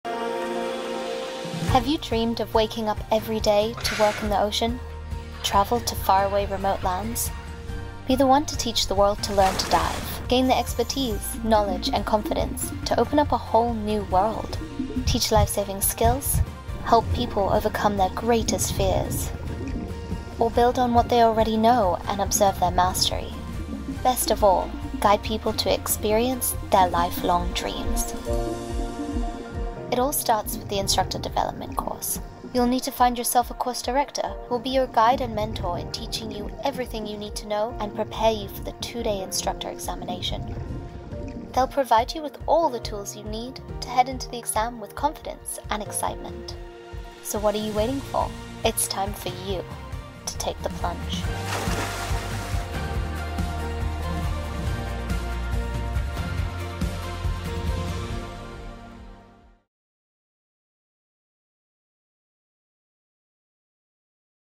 PADI Instructor Development Course (IDC) - Become a PADI Pro!
If you like people, have a passion for scuba diving and want an extraordinary life - become a PADI Instructor. Teaching scuba diving allows you to share your love of the aquatic world with others while doing what you enjoy - being in, around and under water.

PADI Open Water Scuba Instructors and PADI Assistant Instructors are the most sought-after dive professionals around the world because they've completed the program that sets the standard for training dive professionals. You earn a PADI Instructor rating through hard work and commitment, but you're rewarded with a job that lets you share incredible underwater adventures with others - transforming their lives for the better and enriching yours.

Are you ready to join the ranks of the dedicated professionals who teach the world's most progressive and popular scuba diver education programs? Then Go PROSM with an Instructor Development Course.

📷 Video credit: @ExpeditionDrenched